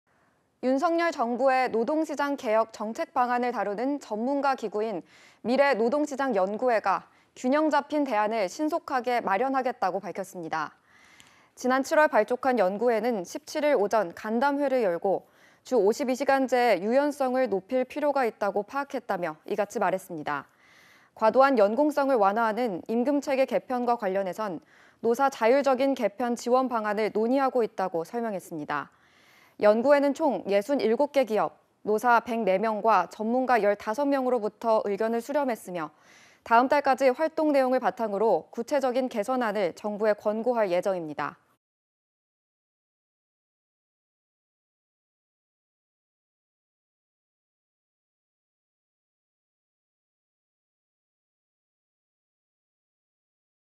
"주52시간·임금 유연성 높여야…균형잡힌 대안 준비"
윤석열 정부의 노동시장 개혁 정책방안을 다루는 전문가 기구인 '미래노동시장 연구회'가 "균형 잡힌 대안을 신속하게 마련하겠다"고 밝혔습니다.
 지난 7월 발족한 연구회는 어제(17일) 오전 간담회를 열고 " 주52시간제의 유연성을 높일 필요가 있다고 파악했다"며 이같이 말했습니다.
 과도한 연공성을 완화하는 임금체계 개편과 관련해선 노·사 자율적인 개편 지원방안을 논의하고 있다고 설명했습니다.
 연구회는 총 67개 기업, 노·사 104명과 전문가 15명으로부터 의견을 수렴했으며 다음달까지 활동 내용을 바탕으로 구체적인 개선안을 정부에 권고할 예정입니다.

🧑🏻진행자 : 박서휘 아나운서